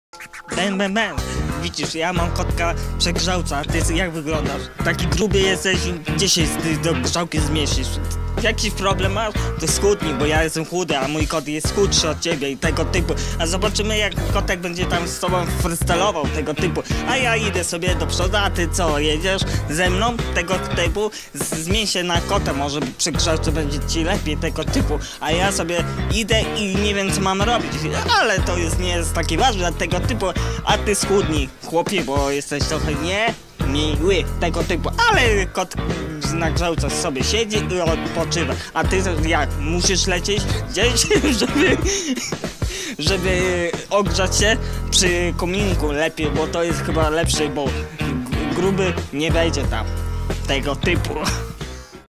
Mój frystal na ocenę do wipka on jest gruby jak ten co go dissuje to diss też na niego
Bec